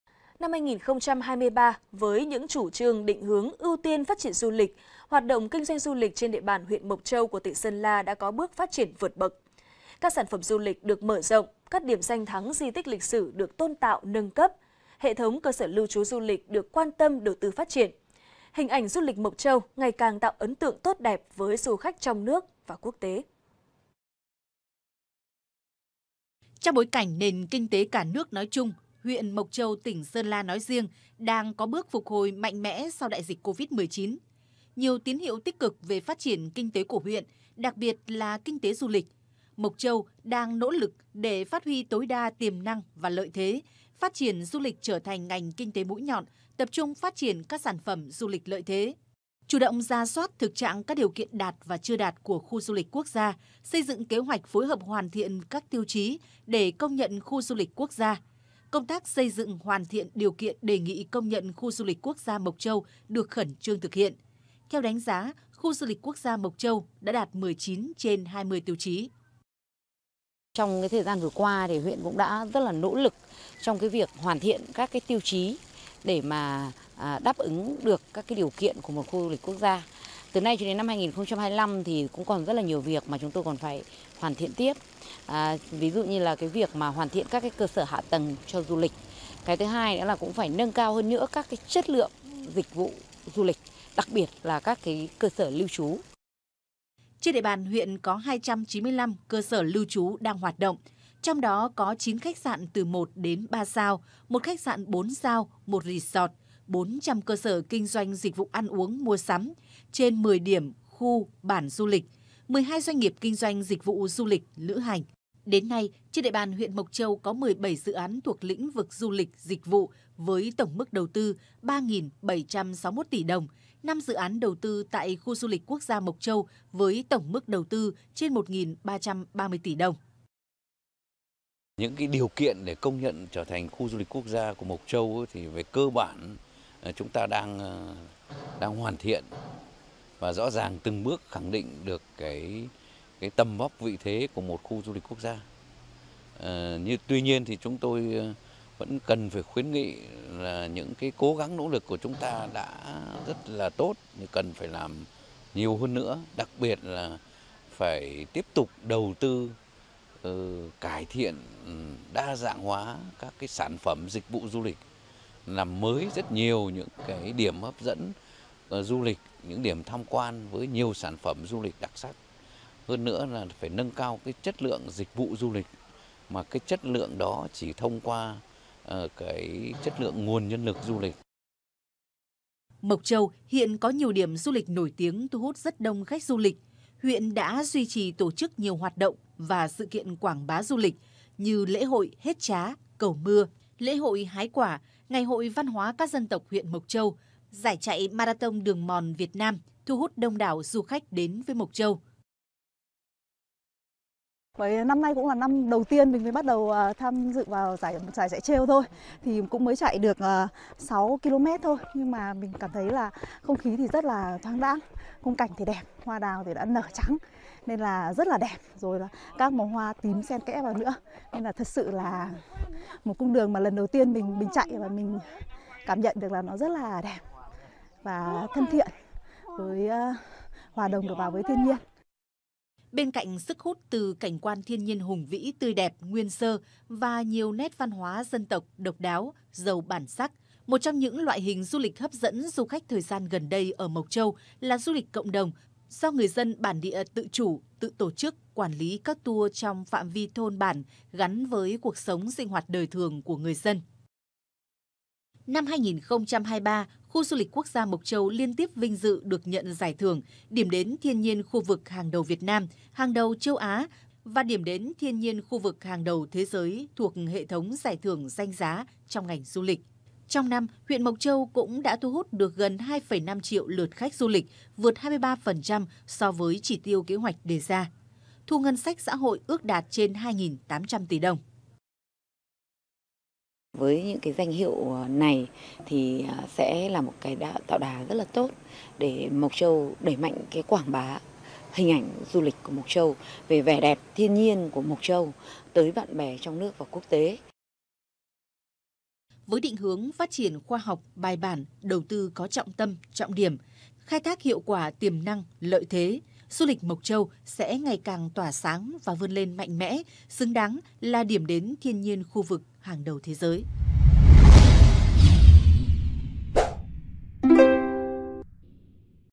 Du lịch Mộc Châu khởi sắc |VTC14 GREEN
VTC14 GREEN| Năm 2023, với những chủ trương, định hướng ưu tiên phát triển du lịch, hoạt động kinh doanh du lịch trên địa bàn huyện Mộc Châu, tỉnh Sơn La có bước phát triển vượt bậc. Các sản phẩm du lịch được mở rộng, các điểm danh thắng, di tích lịch sử được tôn tạo, nâng cấp, hệ thống cơ sở lưu trú du lịch được quan tâm đầu tư phát triển. Hình ảnh du lịch Mộc Châu ngày càng tạo ấn tượng tốt đẹp với du khách trong nước và quốc tế